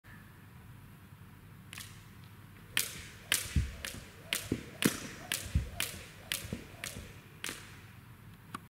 Heel to Toe Ferse vorwärts und Auftippen rückwärts (5)
Die Trendsportart Rope Skipping eignet sich aufgrund seines Spaßfaktors und seiner schnellen Erfolgserlebnisse hervorragend zur Erarbeitung und Präsentation einer Gruppenchoreografie.